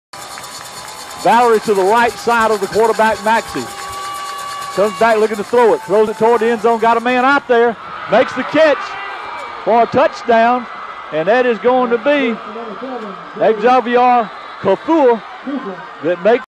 Central Gwinnett #7 Azaviar Kufour catches a 15 yard touchdown pass.
For Full game
access go to
http:NFHSNetwork.com